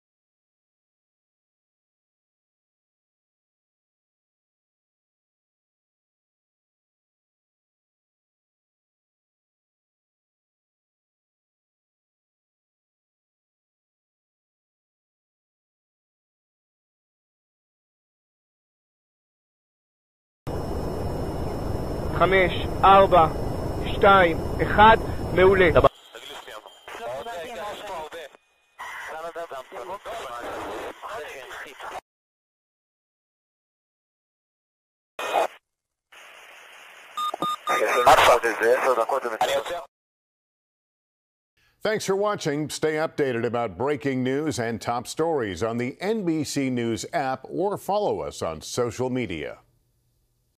Israeli military video shows 'strikes on Hamas military targets' inside the Gaza Strip
The Israeli military released footage of what it described were strikes on Hamas targets in the Gaza Strip, after Hamas militants fired thousands of rockets and sent dozens of fighters into Israeli towns near the Gaza Strip in an unprecedented attack on Saturday.